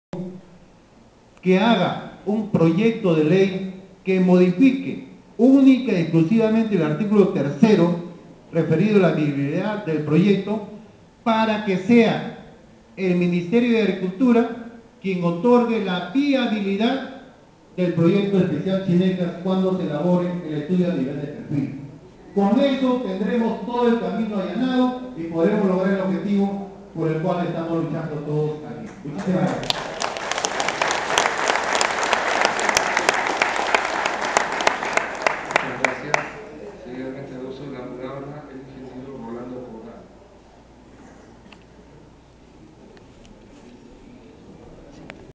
Luis Luna plantea Modificatoria Ley Chinecas.wmv
Esto ocurrio en el salon Grau del Congreso de la Republica el Lunes 17 de Octubre donde se solicito que de modo transitorio se revierta Chinecas al Gobierno Central para que haga un verdadero expediente luego se transfiera a las Juntas de Usuarios